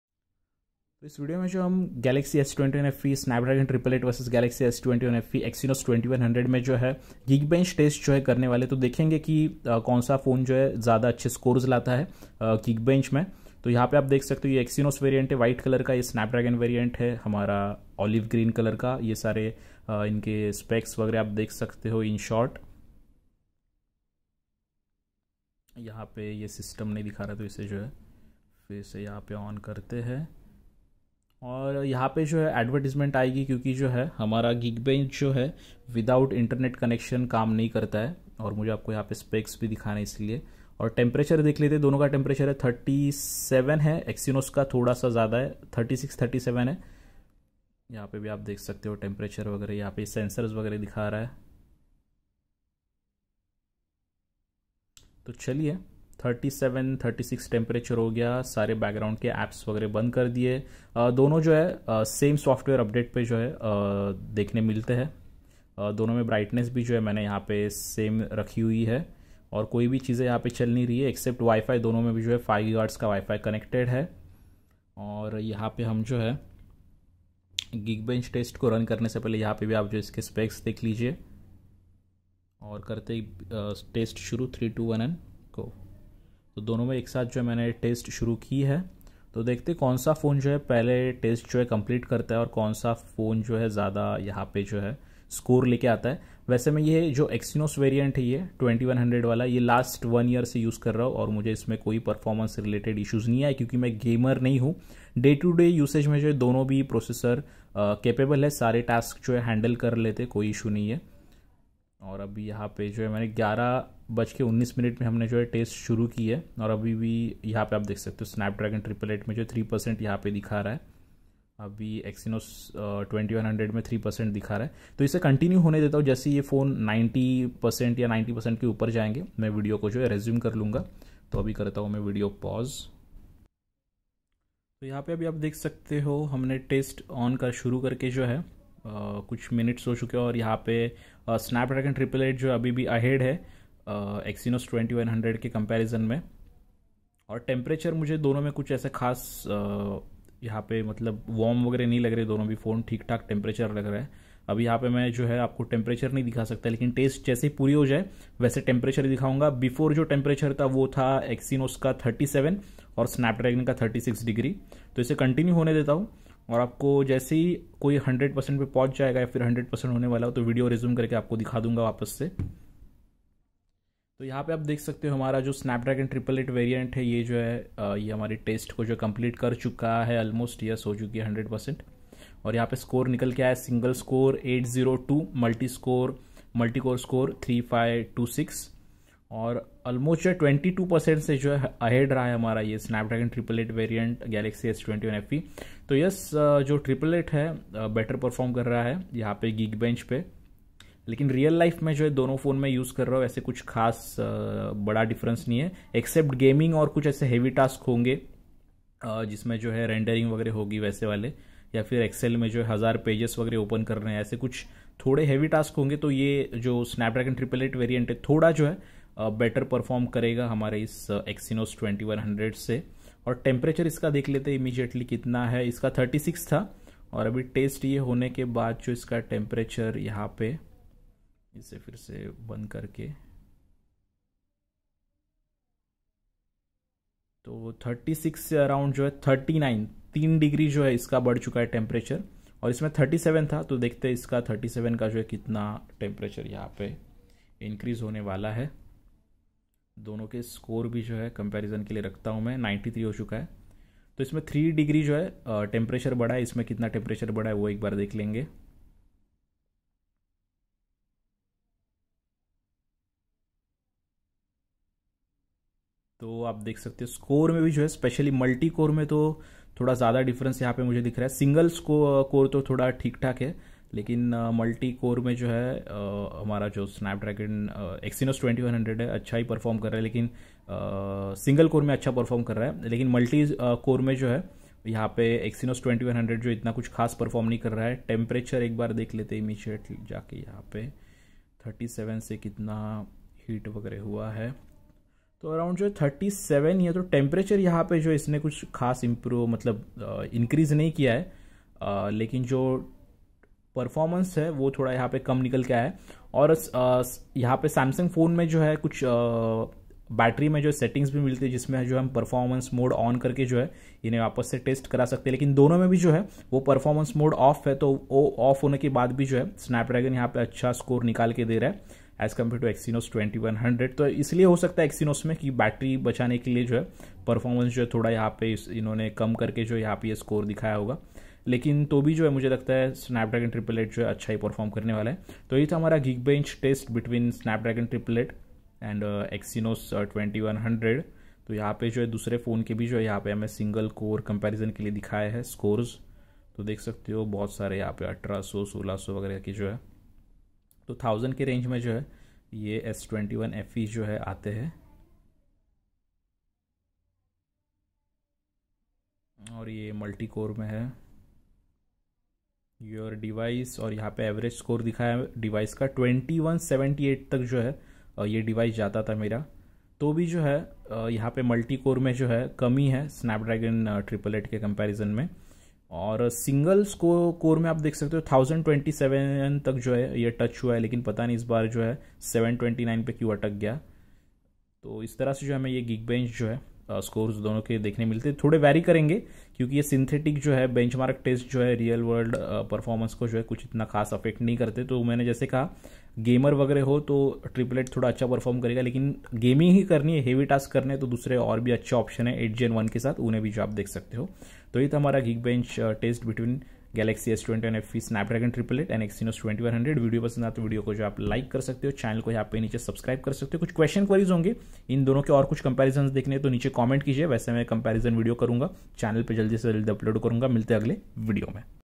Galaxy S21Fe Snapdragon 888 VS Exynos 2100 Geekbench 6 CPU Test | Synthetic Benchmark
Namaskar Dosto,🙏

Toh iss video mai mene aapko Galaxy S21Fe Snapdragon 888 VS Exynos 2100 mai Geekbench 6 CPU Test karke dikhai hai.

Samsung Galaxy S21 FE 5G (Amazon)

Samsung Galaxy S21 FE 5G (Flipkart)

धन्यवाद.!🙏